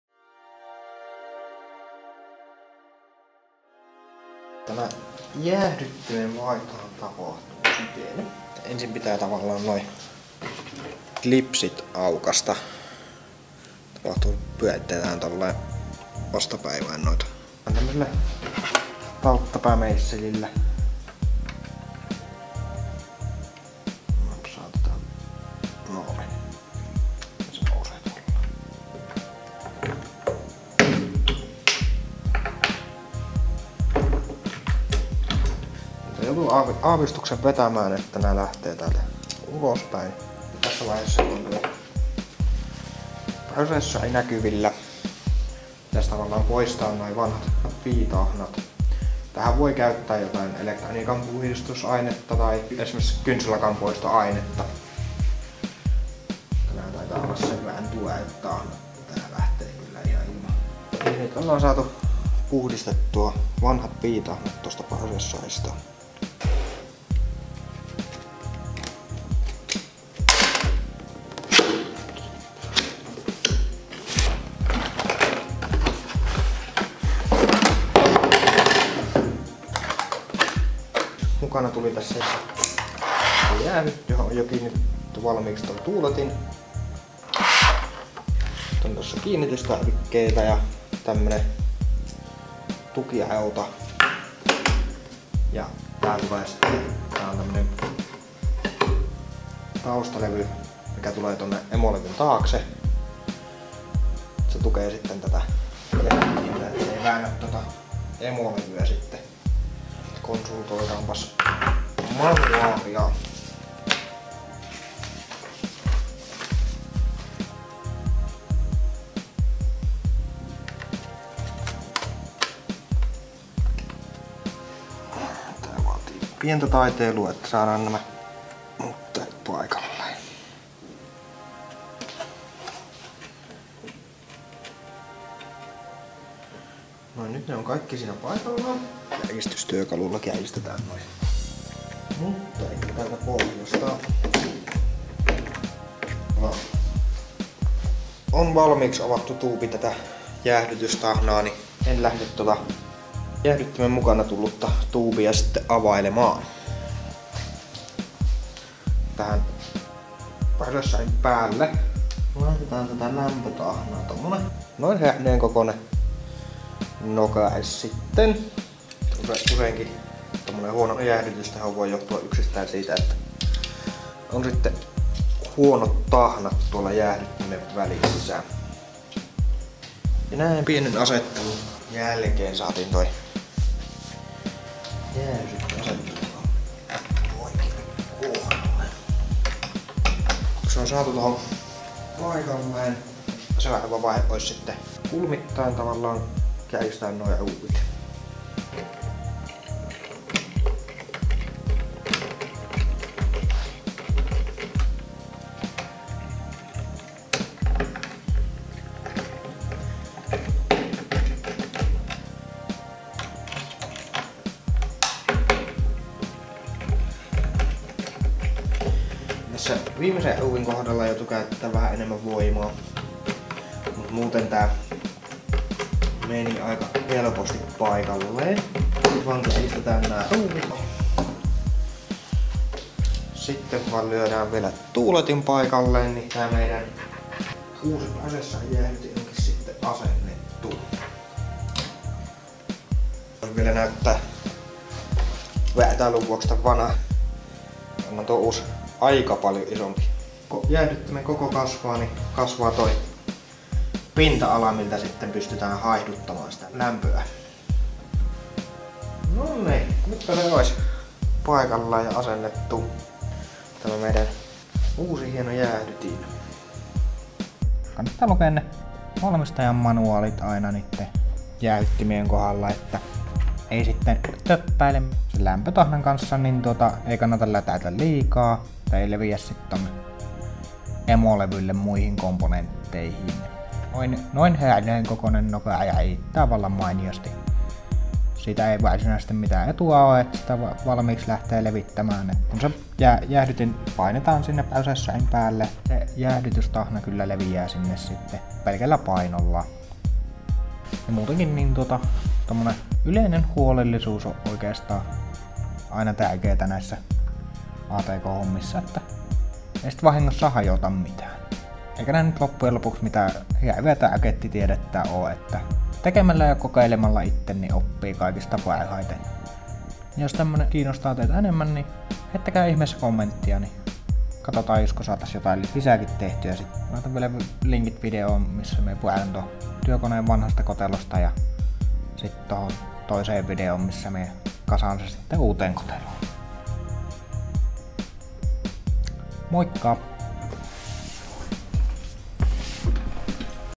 Tietokoneen Intel prosessorijäähdyttimen vaihto - Changing Default Intel Computer CPU Cooler
J-P vaihtaa Intel prosessorin vakiojäähdyttimen Coole Master Hyper 212 EVO-jäähdyttimeen.

0:04 Vakiojäähdyttimen irroitus. / Removing stock cooler.
0:39 Lämpötahnan poisto prosessorin päältä. / Removing old thermal paste from processor.
1:19 Uuden jäähdyttimen asennus. / Installing new cooler.